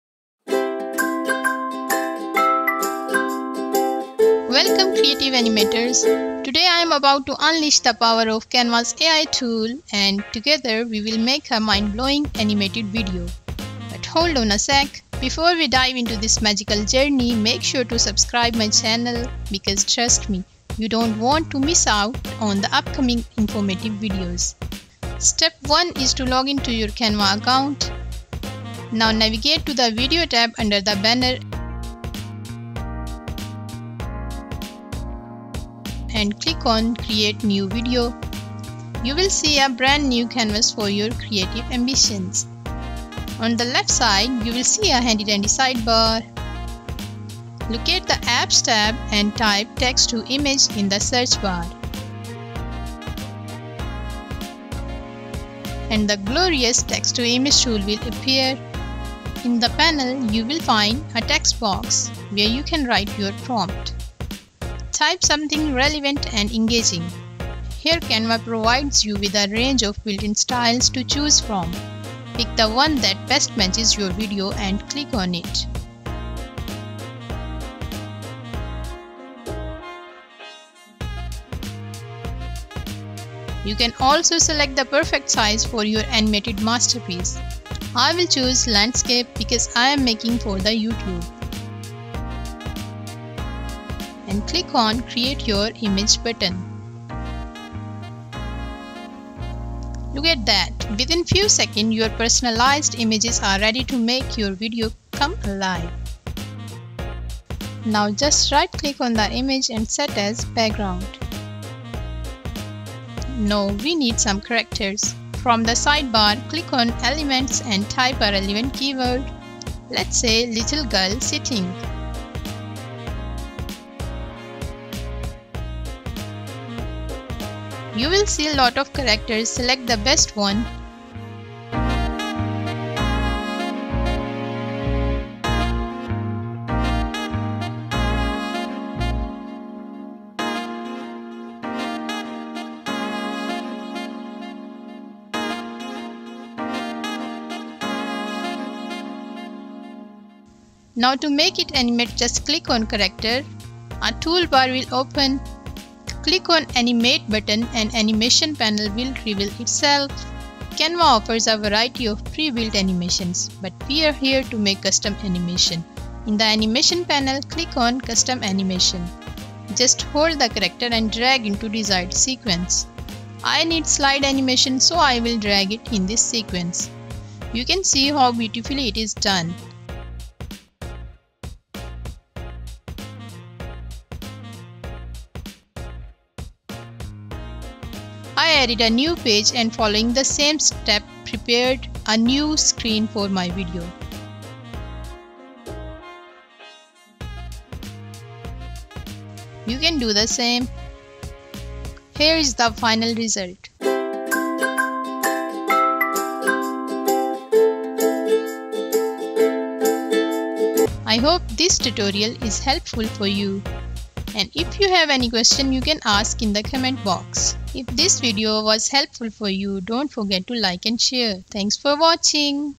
Create animation with AI in 5 minutes | Create animation using canva | Easy tutorial
Experience the magic of Canva's latest AI Tools as we effortlessly bring you this stunning animation. With the power of Canva and the newly released AI Tools, we've crafted a beautiful animation with minimal effort. Free and Pro users can now enjoy the Click and Drag of an Element animation feature, allowing you to add captivating motion to your designs. Keep in mind that each animation is limited to a maximum duration of 10 seconds, ensuring dynamic and concise visual storytelling. Additionally, you can create up to 50 animations within a single design, unleashing your creativity like never before. Explore the limitless possibilities of Canva's AI-driven animation tools and unlock a world of animated brilliance.